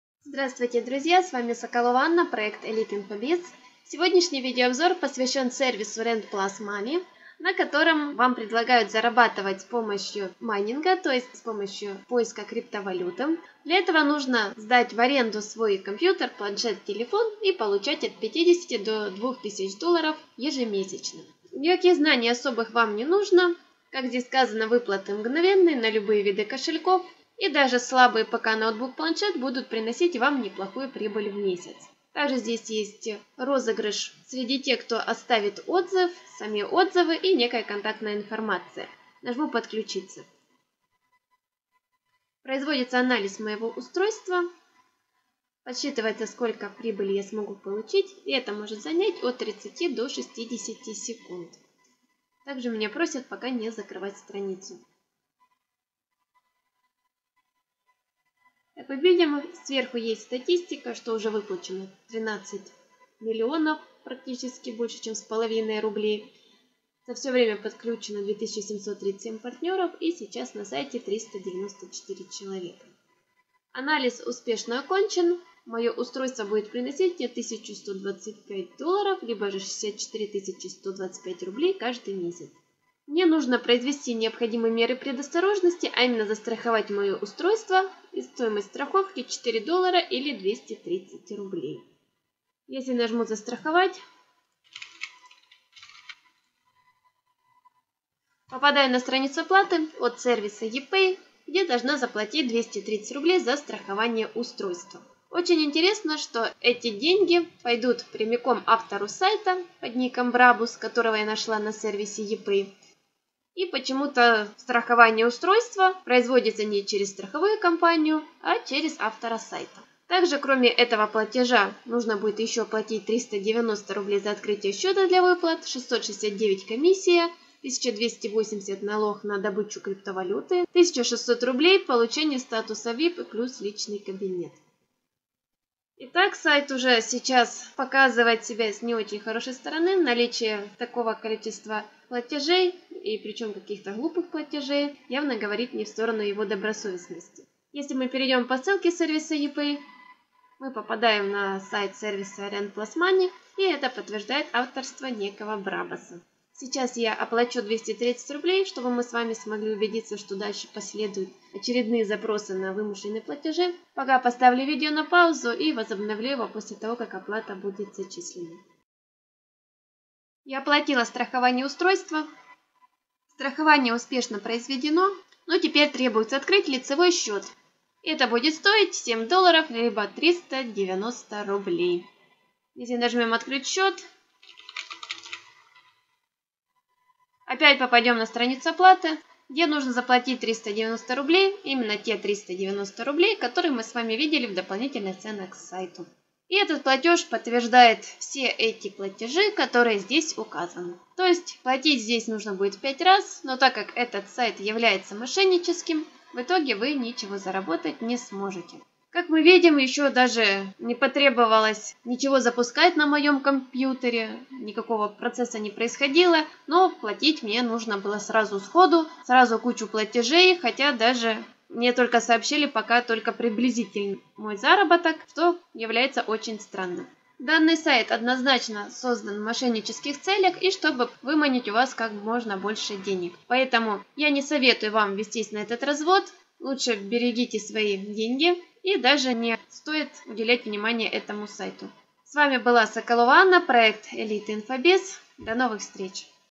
Сервис Rent+Money платит от 50 до 2000$ за аренду вашего устройства для майнинга? Честный отзыв.
На мошенническом сайте сервис Rent+Money должен был заплатить нам 1125$ за аренду нашего ПК. Но не успели мы узнать сколько нам причитается, как на нас посыпались запросы на различные платежи. Смотрите наш обзор и не попадайтесь на этот лохотрон!